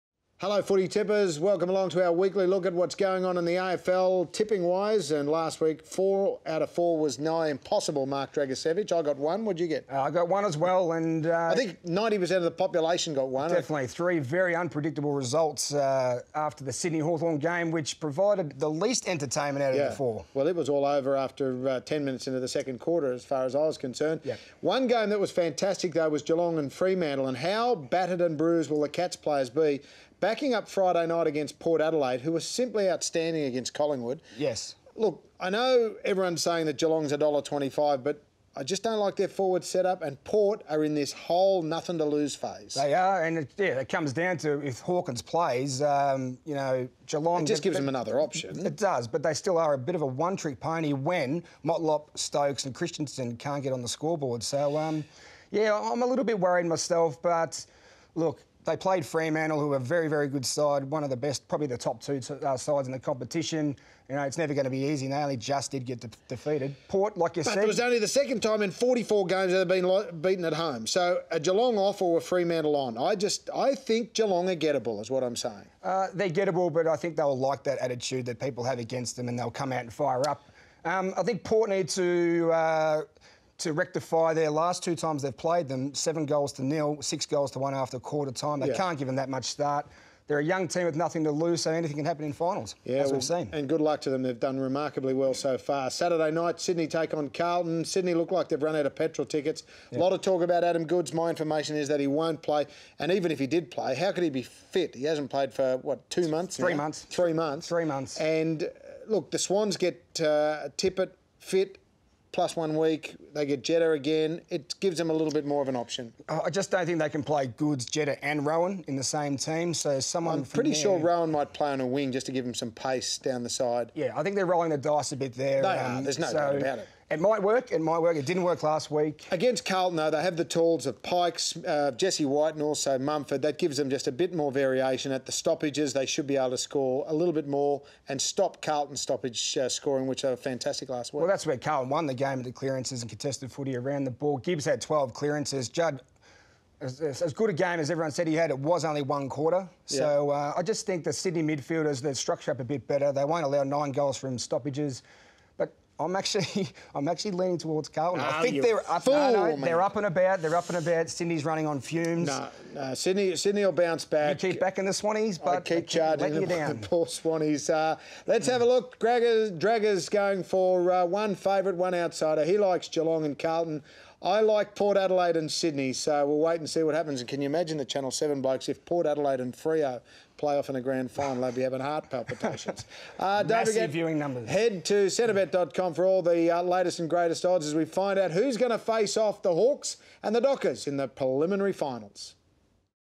AFL Semi Finals Preview
Russell Barwick of ESPN's PTI and Marc Dragicevic preview the second week of the AFL finals.